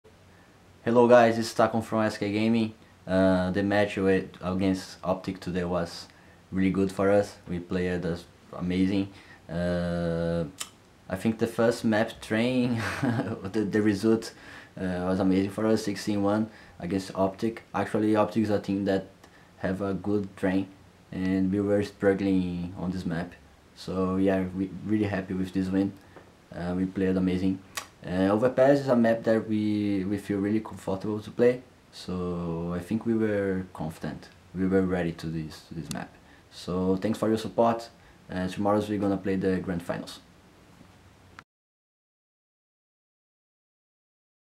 TACO Semifinals Review @IEM Sydney
Taco reviews their semifinals performance at IEM Sydney.